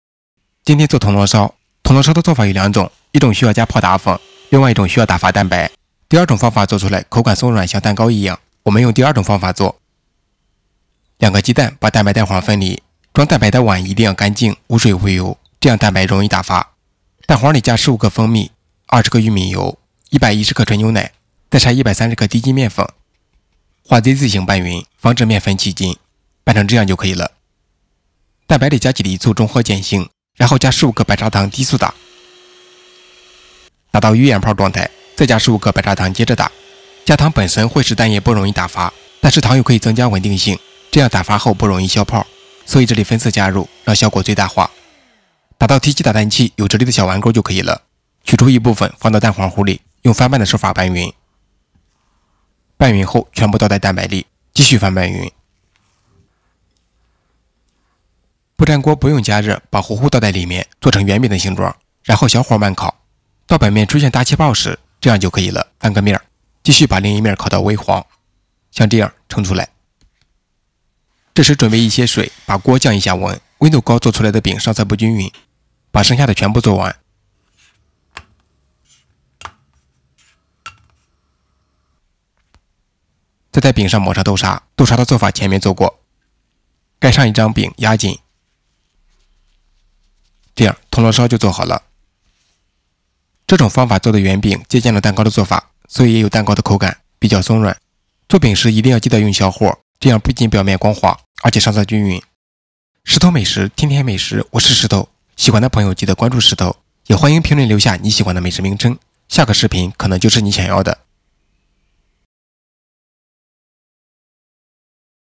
【石头美食】铜锣烧用平底锅就能做！这样制作表面光滑 上色均匀 松软香甜！
Dorayaki Recipe/How to make Dorayaki
不到2分钟学做铜锣烧！用平底锅就能做，方法超简单！
这样制作出来的铜锣烧表面光滑，上色均匀，松软又香甜！

【食材配料清单】：
鸡蛋：2个   蜂蜜：15g   玉米油：20g   纯牛奶：110g
低筋面粉：130g   白砂糖：30g   豆沙：300g
【详细做法】：
1、两个鸡蛋，把蛋白蛋黄分离，装蛋白的碗要无水无油，这样蛋白容易打发
2、蛋黄里加15g蜂蜜，20g玉米油，110g纯牛奶，再筛130g低筋面粉，拌匀
3、蛋白加几滴醋、15g白砂糖低速打，打到有鱼眼泡，再加15g白砂糖接着打
4、打到提起打蛋器有直立的小弯钩，取出一部分放到蛋黄糊里，用翻拌的手法拌匀
5、拌匀后全部倒在蛋白里，继续翻拌匀
6、不粘锅不用加热，把糊糊倒在里面，做成圆形的形状，然后小火慢烤
7、到表面出现大气泡时，，翻个面，继续把另一面烤到微黄，烤好盛出来
8、准备一些水，把锅降一下温，温度高做出来的饼上色不均匀，把剩下的全部做完
9、再在饼上抹上豆沙，豆沙的做法前面做过，盖上一张饼，压紧就可以了
大家好，欢迎来到【石头美食】
石头美食，天天美食！